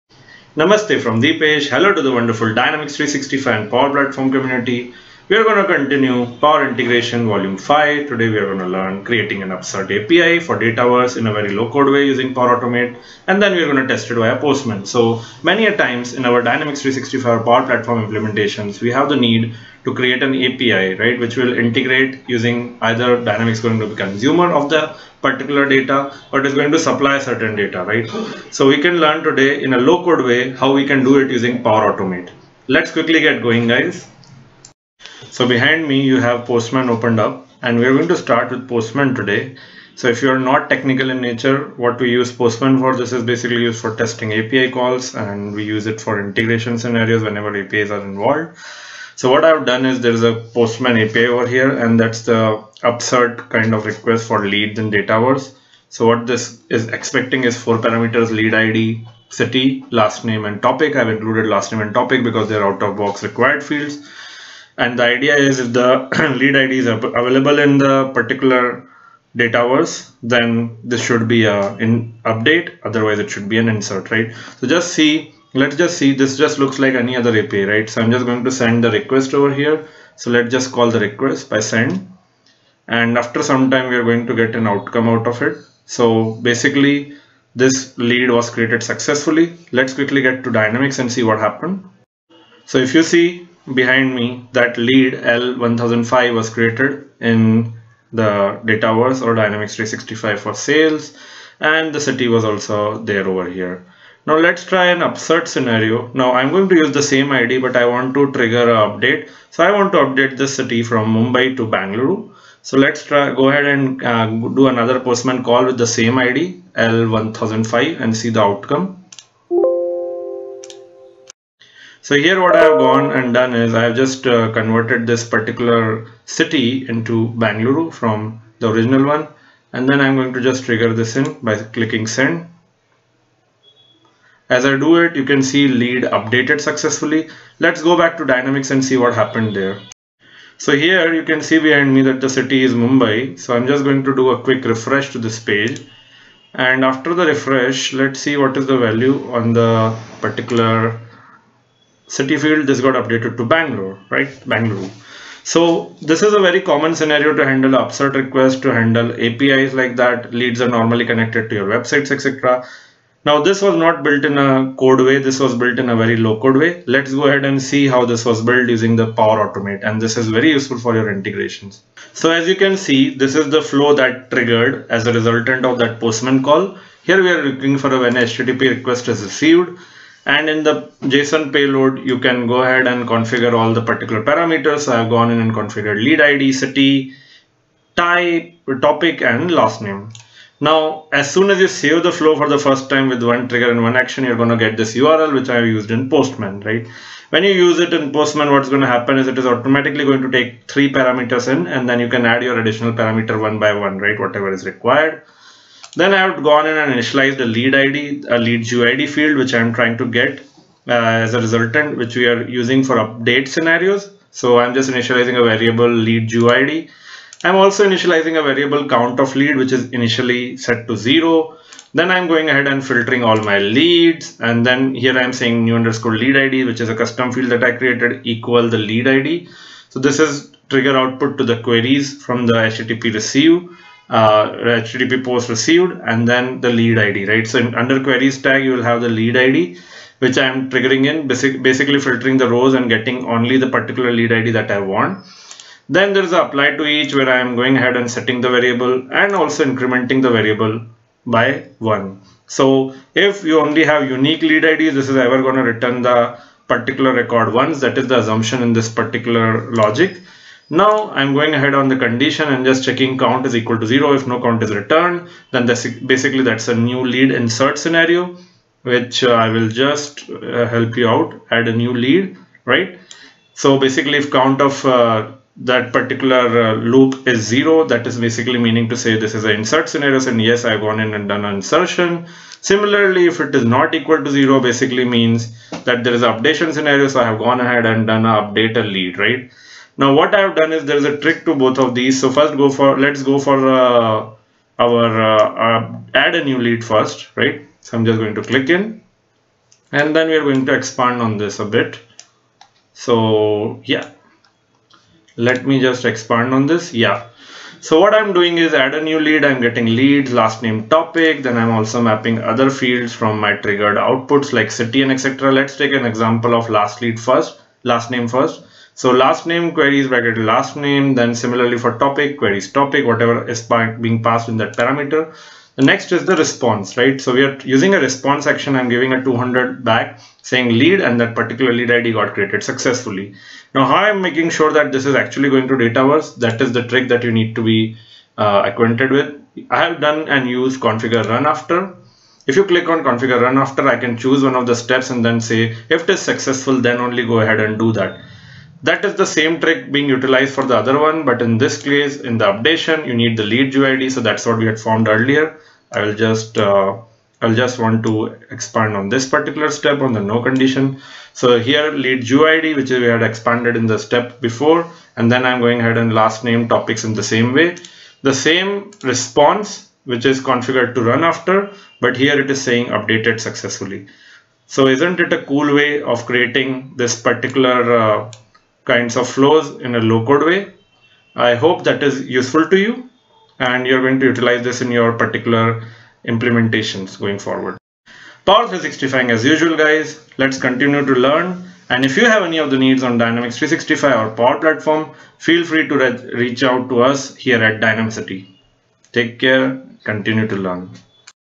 Power Integration Volume 5: Create Upsert API for Dataverse in a low code way using Power Automate
Power Integration Volume 5: Create an Upsert API for Dataverse in a low code way using Power Automate and test it via Postman